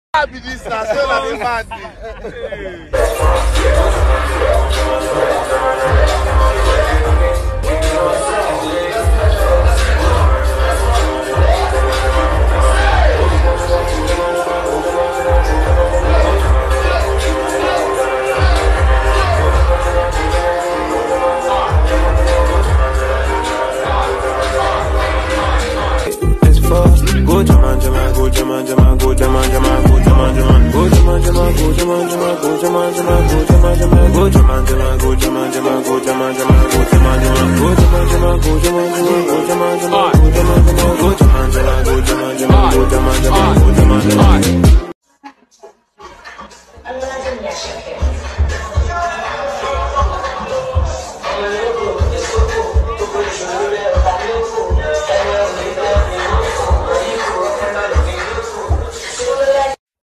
Burna Boy and Big Brother Naija star, Nengi spark dating rumour after being spotted at the club
Burna Boy and Nengi BBNAIJA
Burna Boy is dating Nengi
Nengi Big bro Naija and Burna Boy spotted together.